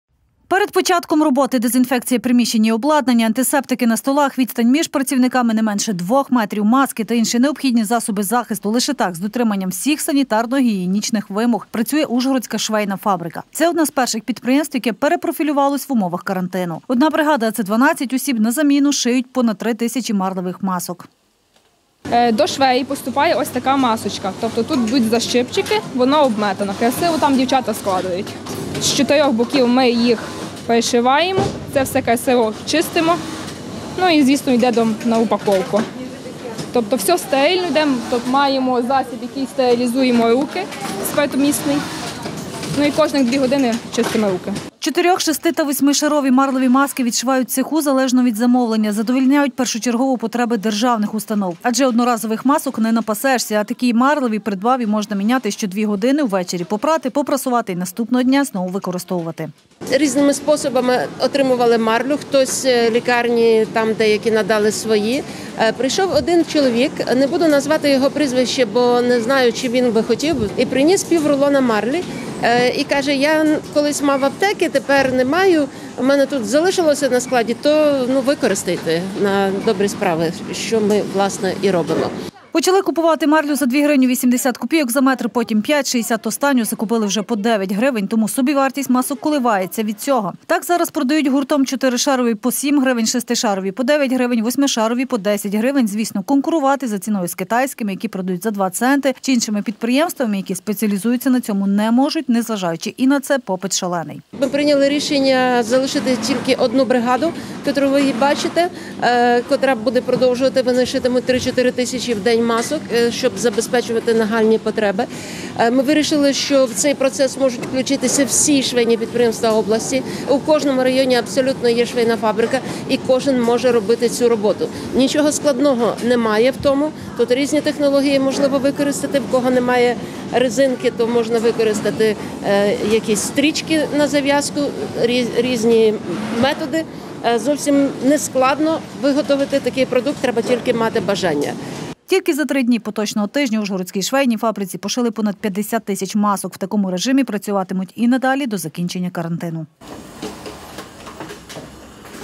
Перепрофілювалися
Три тисячі марлевих масок  шиє бригада за зміну! ПрАТ «Ужгородська швейна фабрика»- одне з перших підприємств, яке перепрофілювалося і умовах карантину. Перед початком роботи - дезинфекція приміщень і обладнання, антисептики на столах, відстань між працівниками не менше 2 м, маски та інші необхідні засоби захисту - лише так,з дотриманням усіх санітарно-гігієнічних  вимог,   працює Ужгородська швейна фабрика. Це одне з перших підприємств, яке перепрофілювалося і умовах карантину. Одна бригада- а це 12 осіб- за заміну шиють понад три тисячі марлевих масок.Чотирьох, шести та восьми шарові марлеві маскивідшивають у цеху, залежно від замовлення. Задовольняють першочергово потреби державних установ. Адже одноразових масок не напасешся, а такі марлеві - кілька придбав і можна міняти кожні дві години, а ввечері попрати, попрасувати і наступного дня знову використовувати.
Починали купувати марлю за 2 гривні 80 копійок за метр, потім п’ять шістдесят, а останню закупили вже по 9 гривень. Тому собівартість масок коливається від. Так, зараз продають гуртом 4 тири шарові по 7 грн, шести шарові по 9 гривень, 8 шарові по десять гривень. Звісно, конкурувати по ціні з китайськими – які продають за 2 цента, чи іншими підприємствами, які спеціалізуються на цьому, не можуть. Незважаючи на це, попит шалений. Тільки за три дні поточного тижня на Ужгородській фабриці пошили понад п’ятдесят тисяч масок. У такому режимі працюватимуть і надалі - до закінчення карантину.